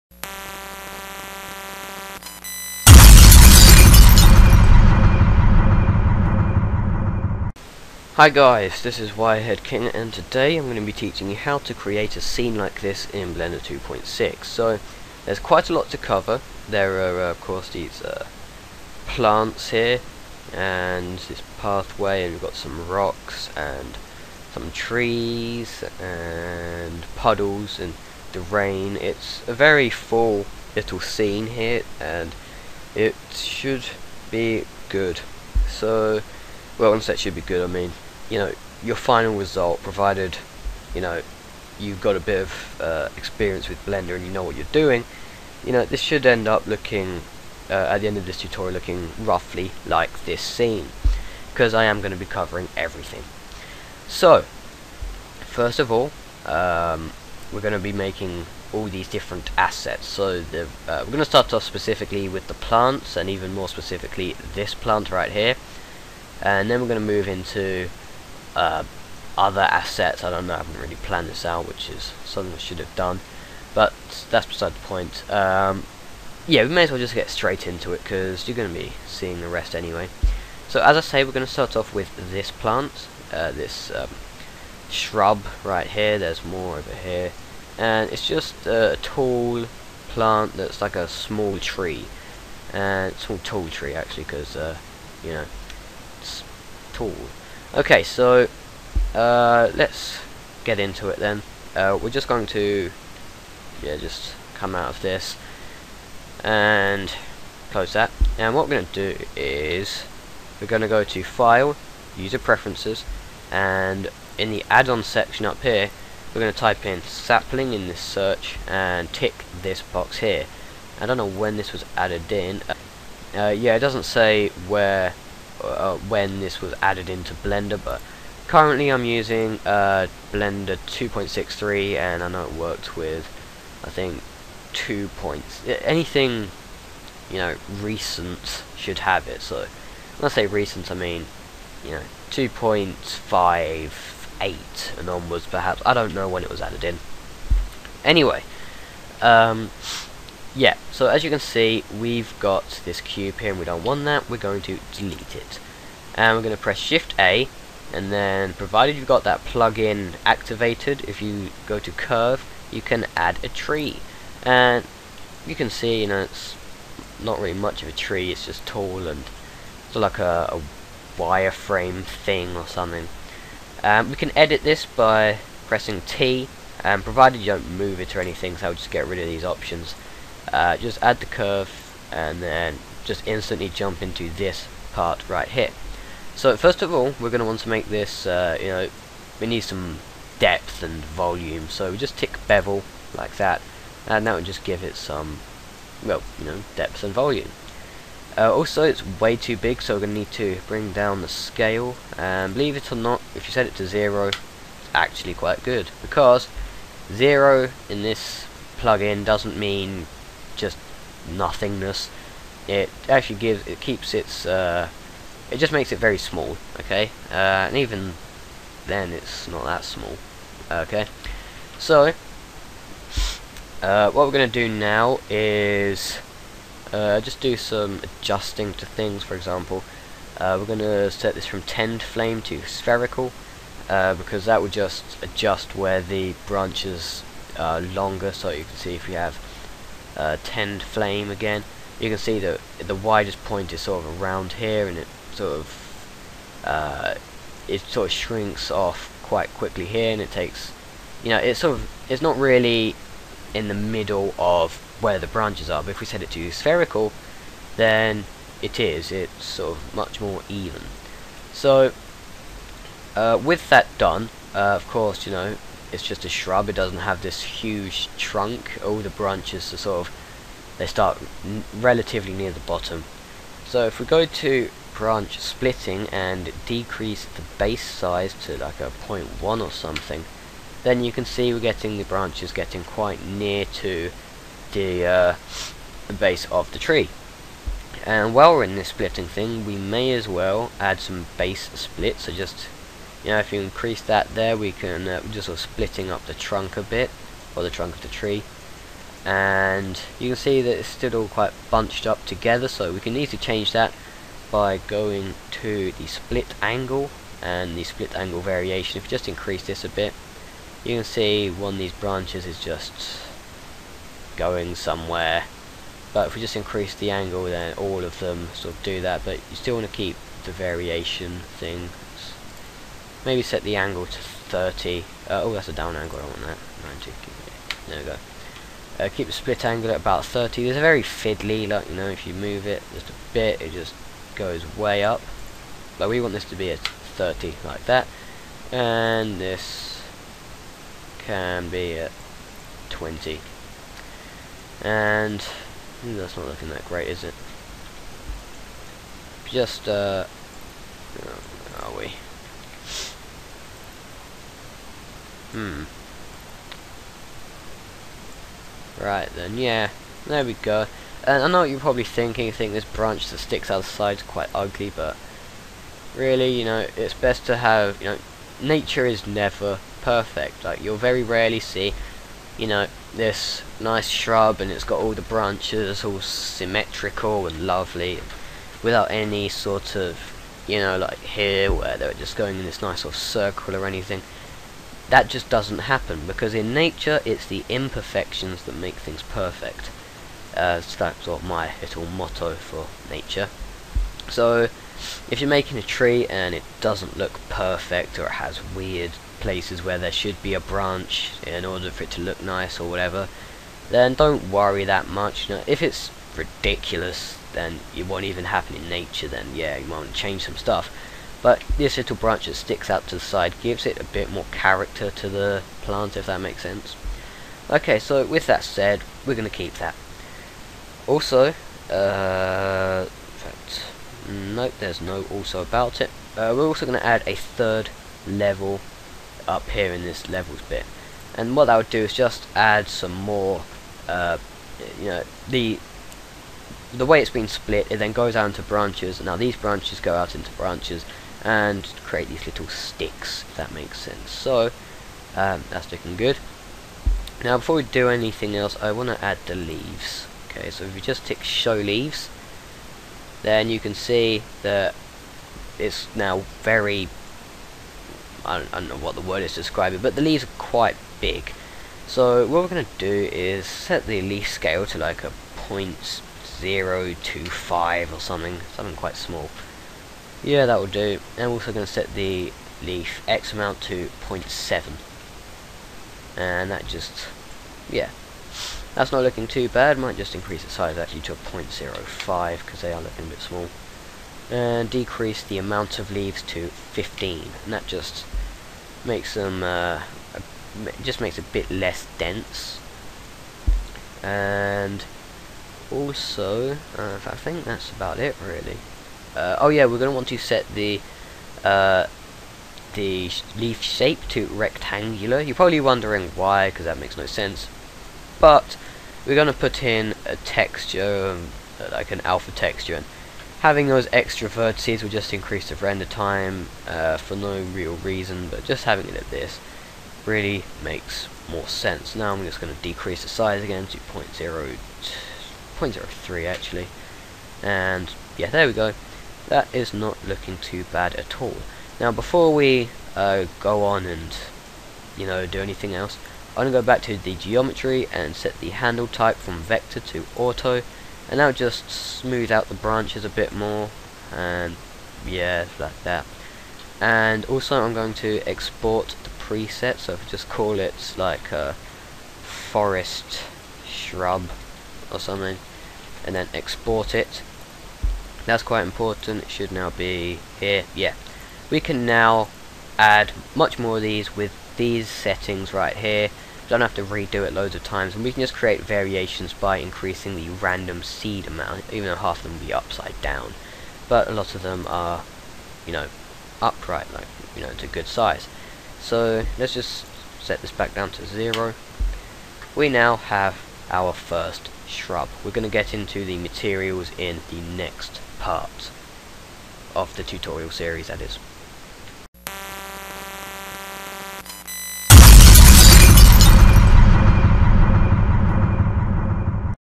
[HD] How to Create a Rainforest in Blender 2.6 (PART #1)
In this tutorial, I will show you how to start creating a rainforest in Blender by creating a simple shrub using the Sapling plug-in that comes built in to Blender.

This series is made up of 16 official parts (but some extentions to the series may come due to requests or personal ideas):